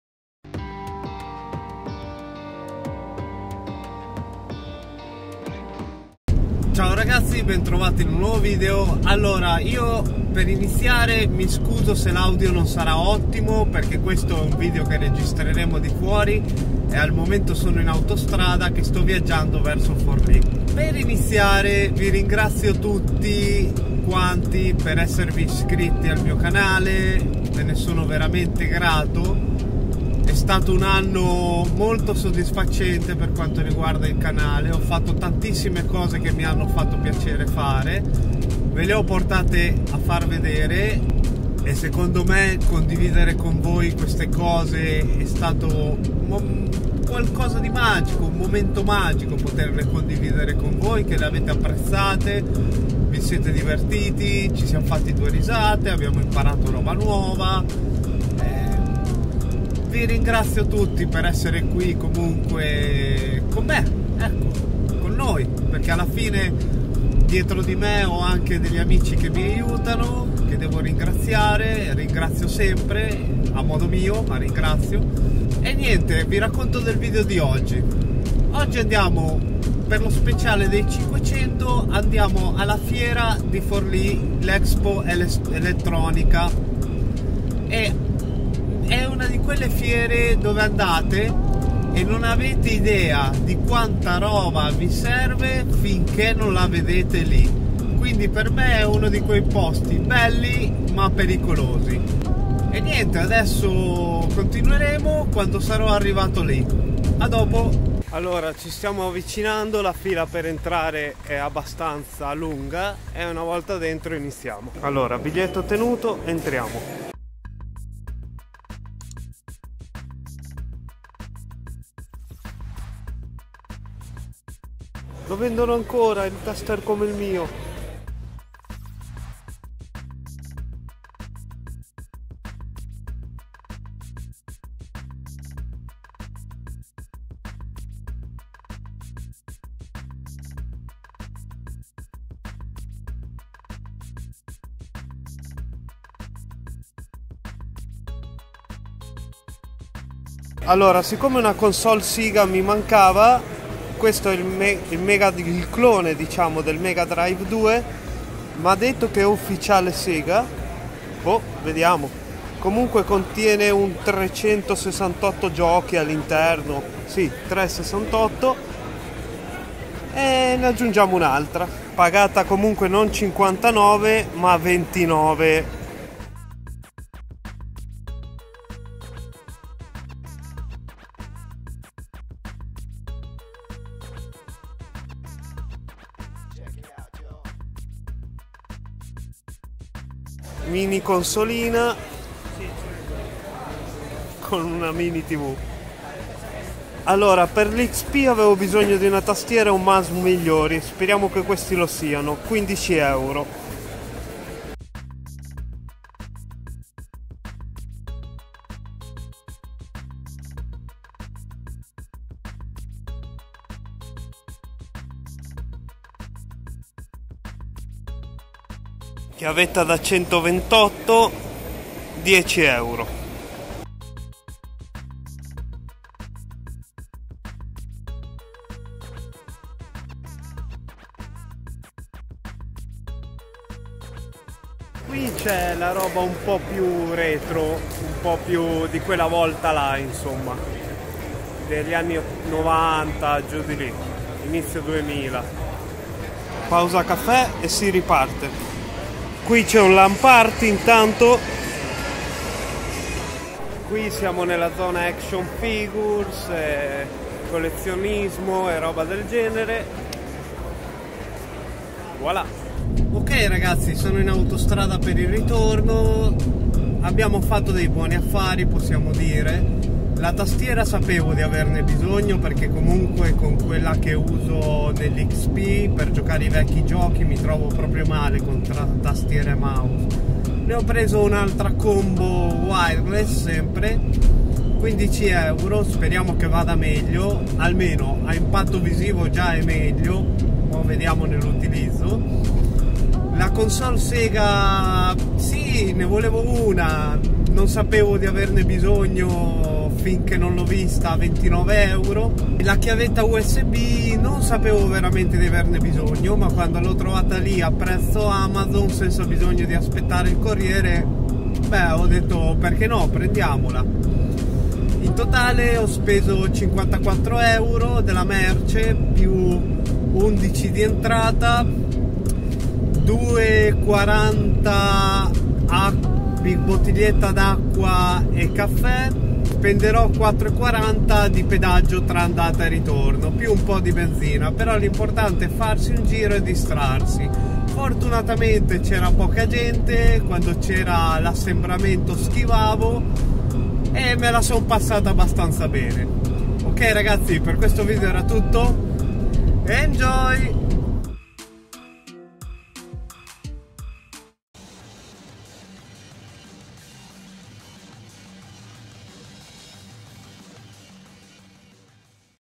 SPECIALE 500 ISCRITTI 😍 - VI PORTO IN FIERA
GRAZIE, GRAZIE, GRAZIE! Vi sono molto grato per questo traguardo, siamo in 500, e per l'occasione ci andiamo a fare un giro a Forlì, all'Expo Elettronica, vediamo se riusciamo a fare buoni affari.

0:00 INTRODUZIONE
1:47 FIERA EXPO ELETTRONICA FORLI
4:22 CONCLUSIONE